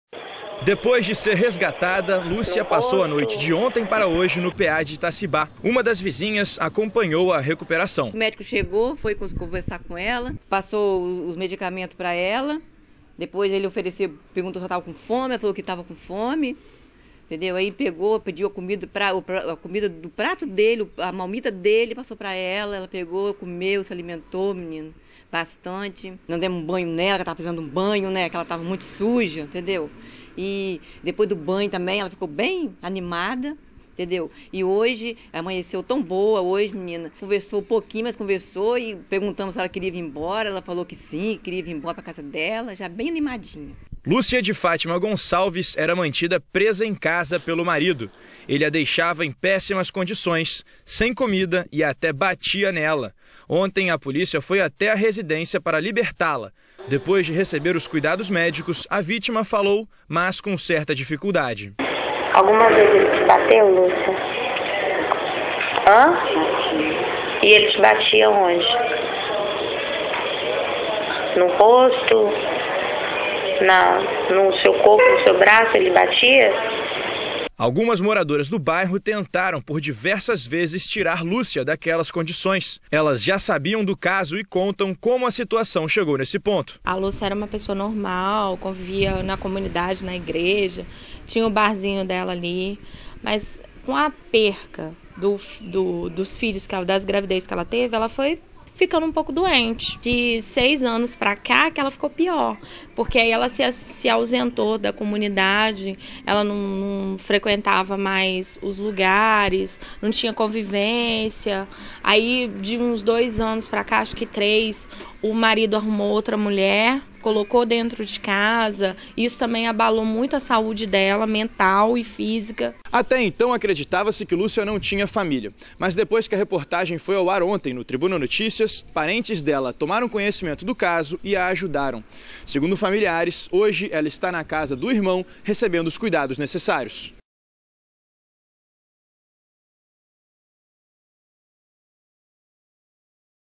Caso - Mulher cárcere privado
Mulher mantida em cárcere privado já teria tido problemas em várias gestações. Em uma gravação ela afirma que apanhava do marido.